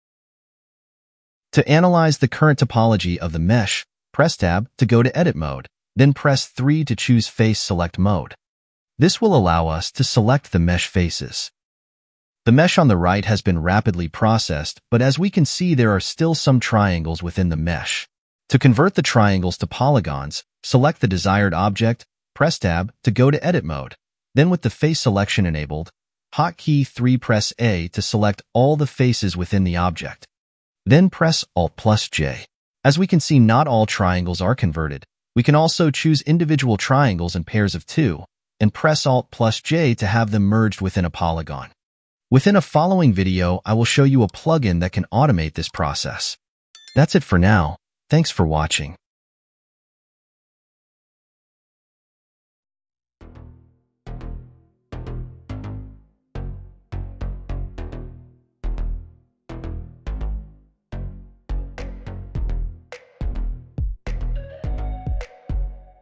Blender how to Convert Triangles Into Quads with no plugins
3D scans usually have the mesh defined by triangles, you can change those within Blender without having to use 3D party plugins.

Making use of various plugins will ensure a faster workflow.